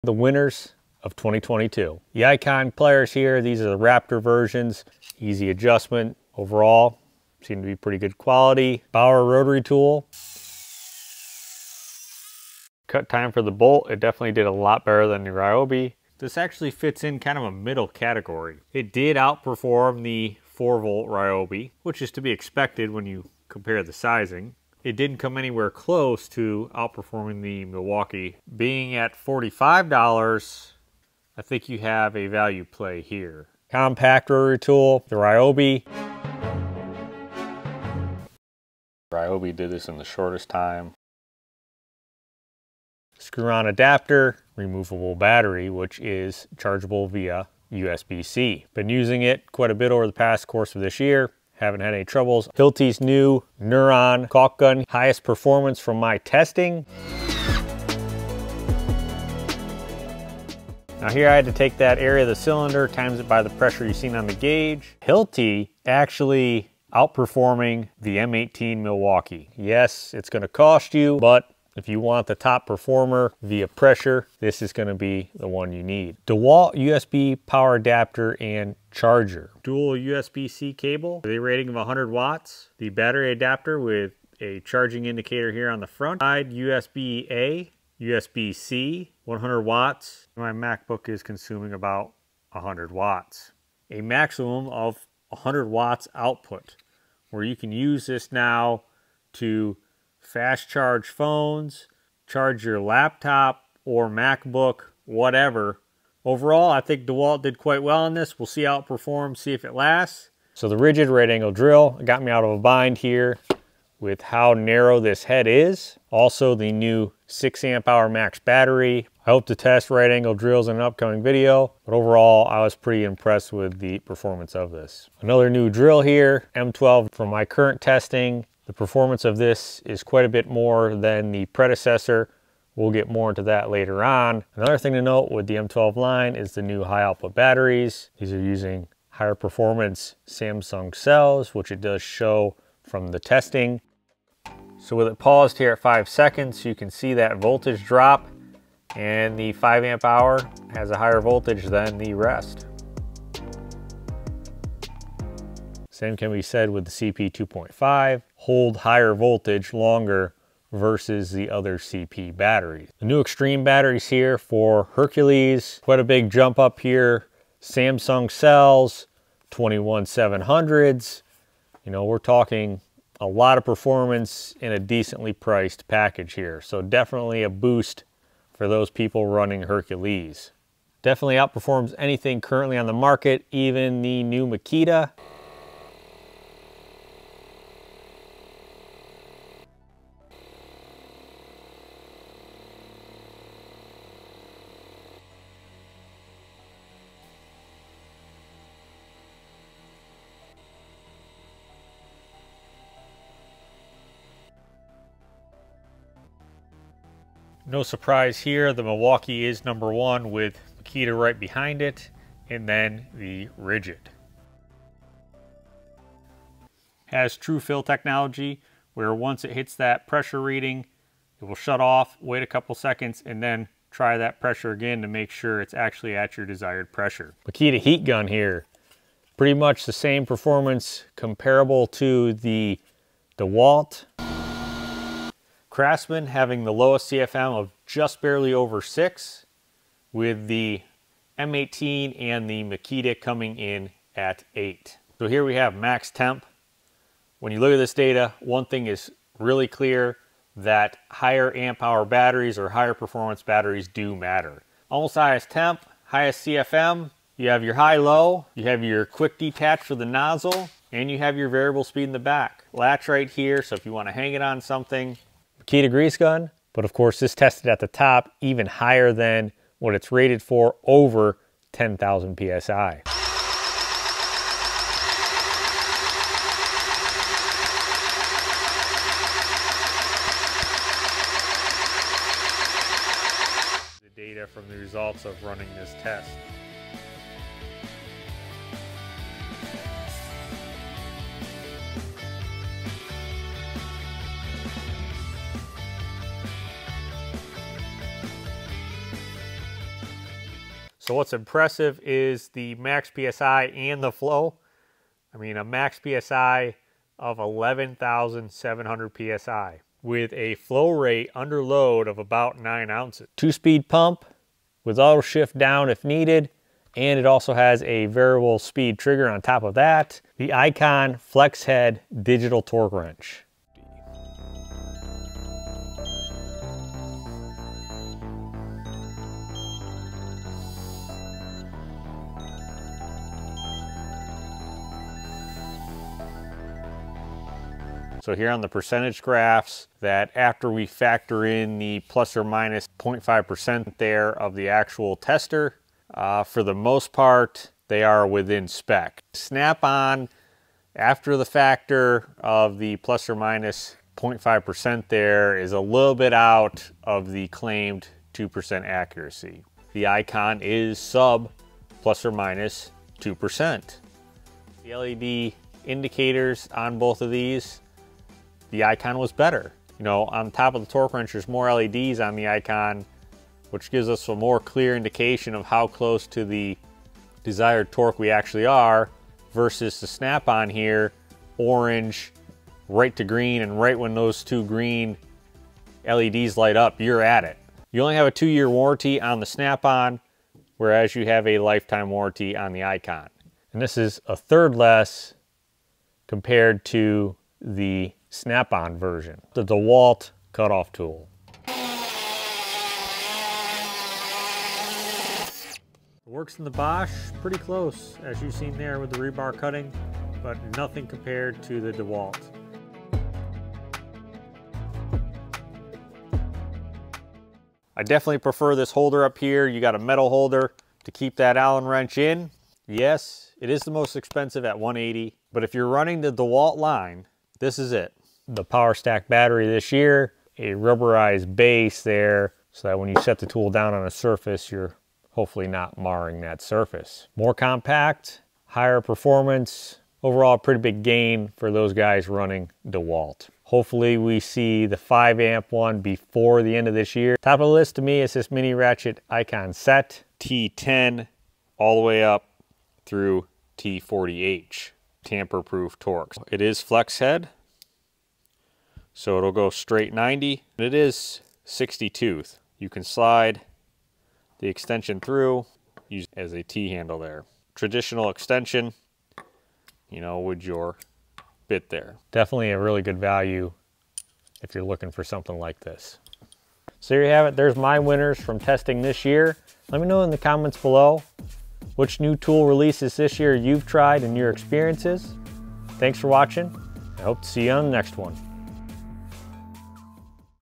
Best New Tools Of 2022 Tested!
New releases of 2022 which out performed the competition from my testing.

📢 M12 Drill,  Right angle drills,  Hercules Extreme Battery videos coming soon!

Product Links

Dewalt DCS438B Cut Off Tool

Makita Grease Gun XPG01Z

Makita Heat Gun XGH01ZK

M18 Inflator 2848-20

M12 Drill 3497-22

Ridgid Right Angle Drill R87701

Dewalt USB Power Adapter/Charger DCB094K

Hilti Caulk Gun 3778710 Ion-Cordless-10-oz-Caulk-Gun-with-Barrel-3778710/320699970

Ryobi 4v Rotary Tool FVM51K

Thank You! for supporting the channel, this helps in me providing new content each week!